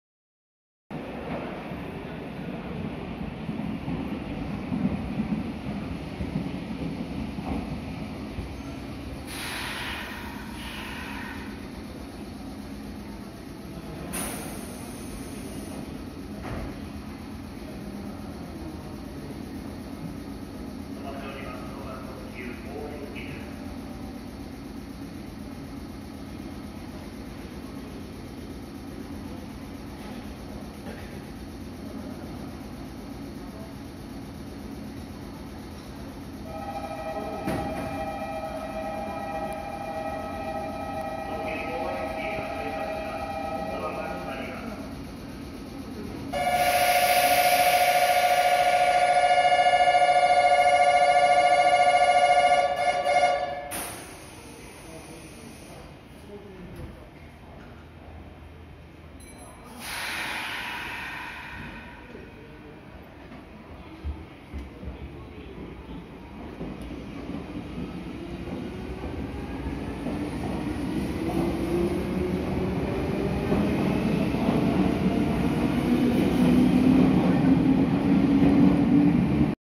名古屋鉄道名鉄６５００系方向幕回転全車一般車特急河和行き日本車輌製造三菱製発車します
携帯電話撮影SONYXPERIAから動画からしました名古屋鉄道名鉄６５００系統方向幕回転全車一般車回送団体特急白幕吉良吉田蒲郡猿投豊田市碧南知立津島佐屋弥富森上金山内海河和みなさまお知らせです名鉄６０００系はお早め撮影してくださいお願いいたしますチャンネル登録お願いいたします再生回数お願いいたします発車します